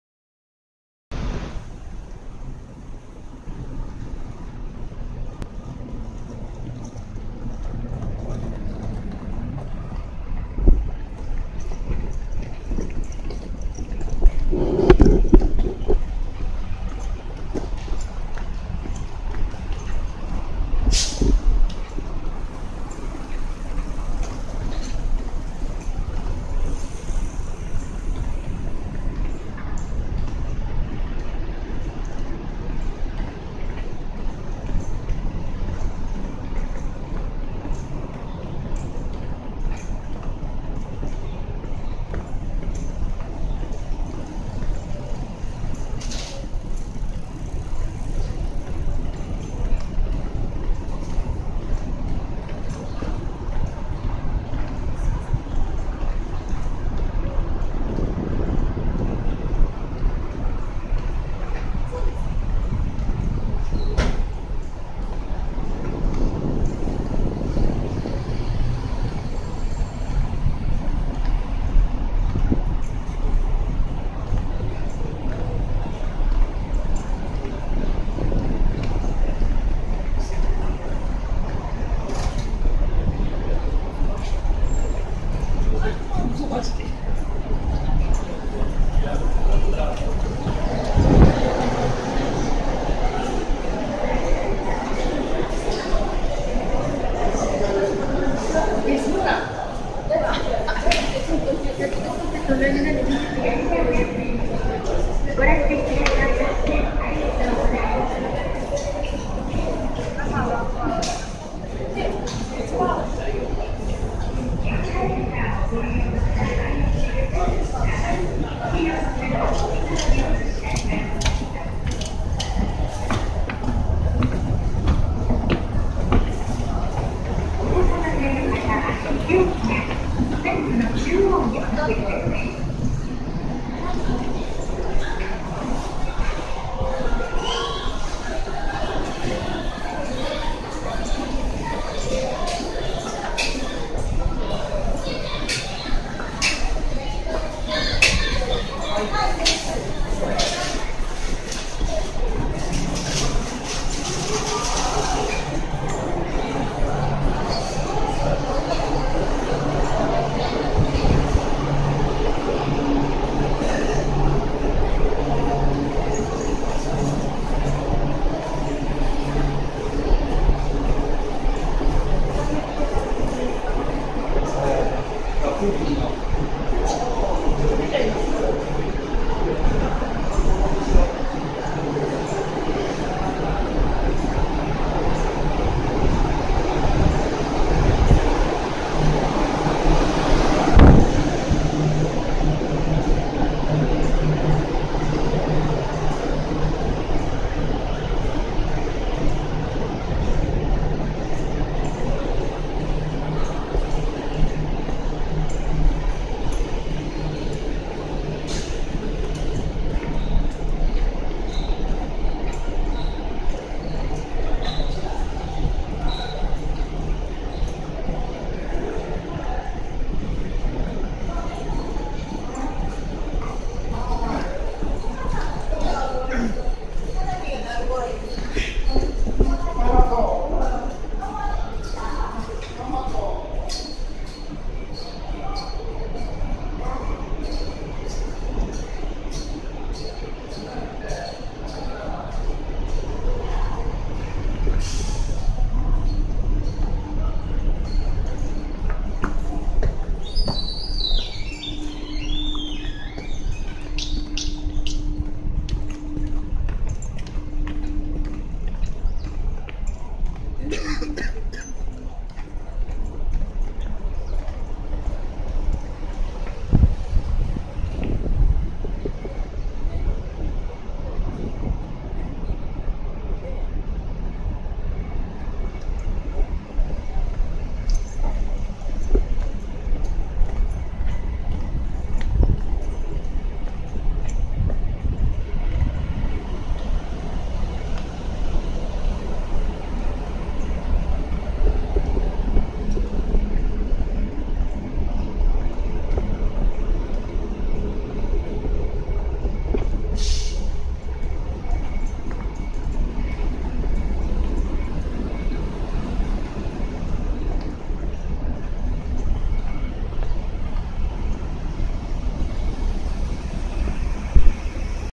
平日夕方の高崎駅
15.8.11
JR高崎駅　群馬県高崎市
ペデストリアンデッキ　駅ナカ
ヤマダ電機 LABI1高崎　イーサイト　モントレー　ビブレ跡地　イオン　改札　構内　東西自由通路
2015年　平成27年